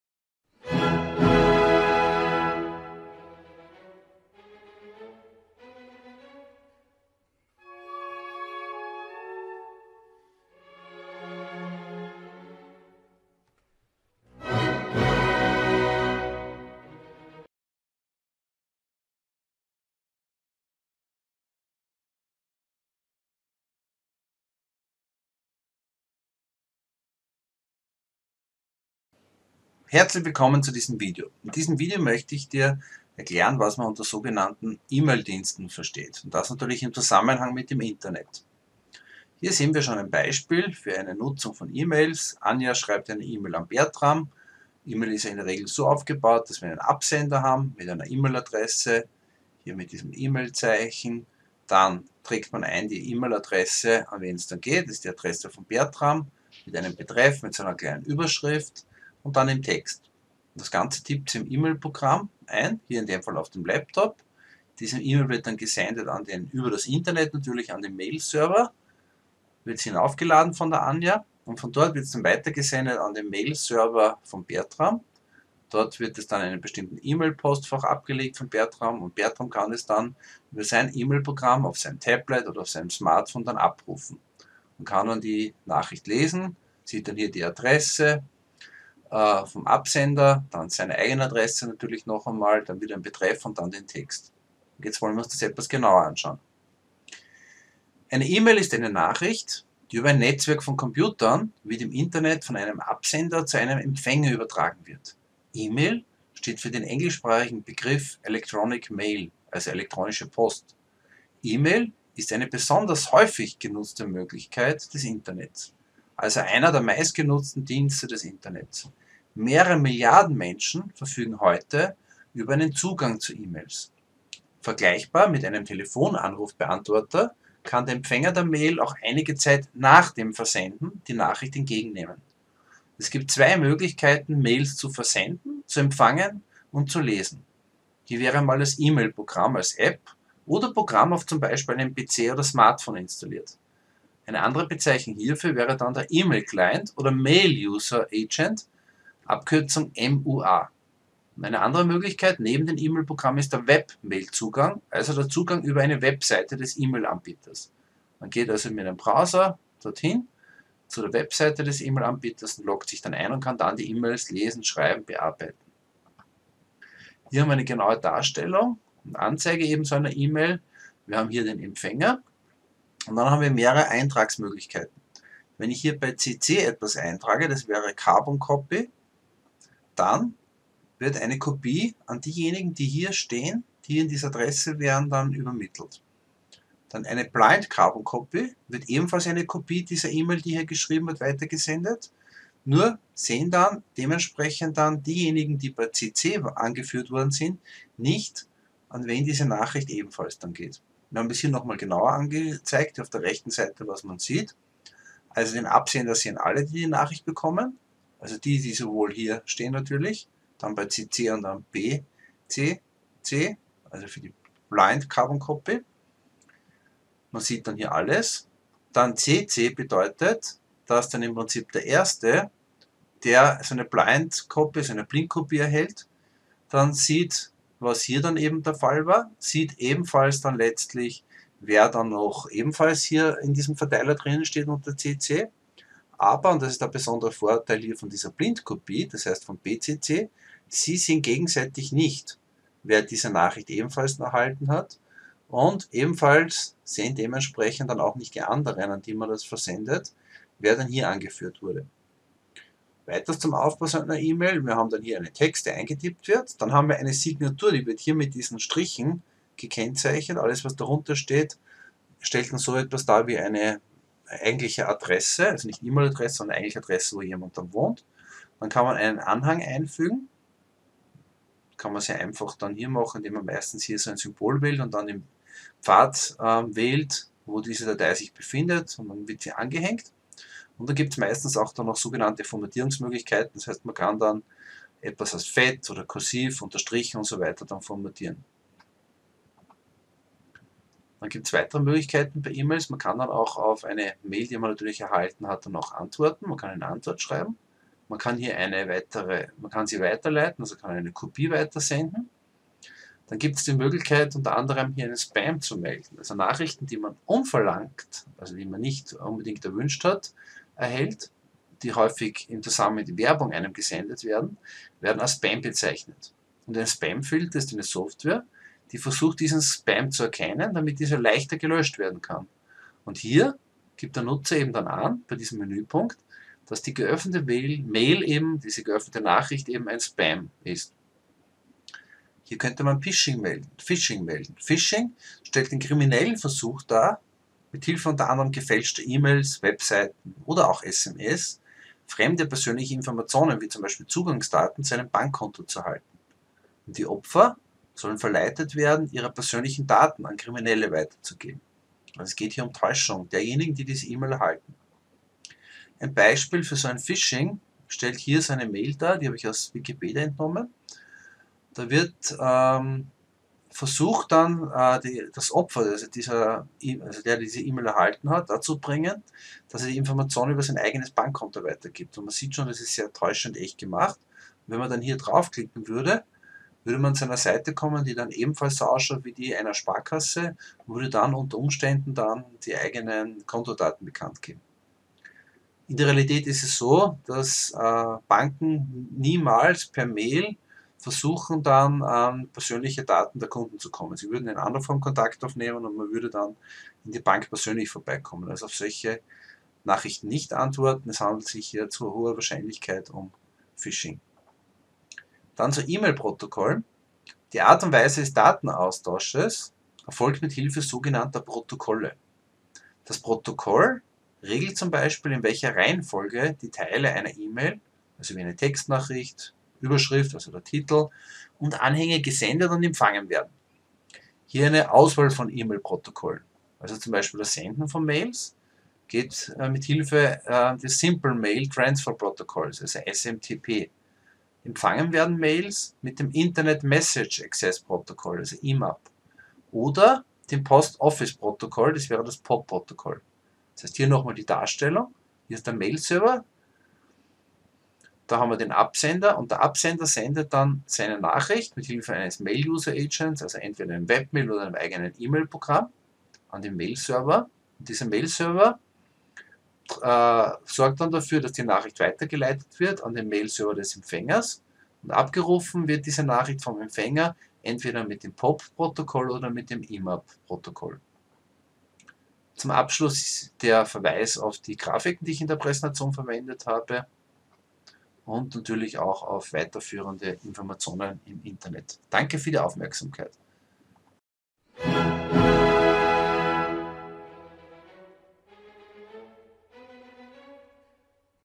E-Mails schreiben und senden - DigiKomp
Es geht darum sich mit den folgenden Inhalten auseinanderzusetzen: Emaildienste nutzen wie Nachrichten verfassen, adressieren an/cc/bcc, senden, weiterleiten, beantworten, Anhänge versenden. Dieses Video ist als Hilfestellung zur Erreichung bzw. Festigung digitaler Kompetenzen von Schüler / innen gedacht - siehe Weitere Unterlagen wie Präsentationen, Quize und andere Videos finden sich und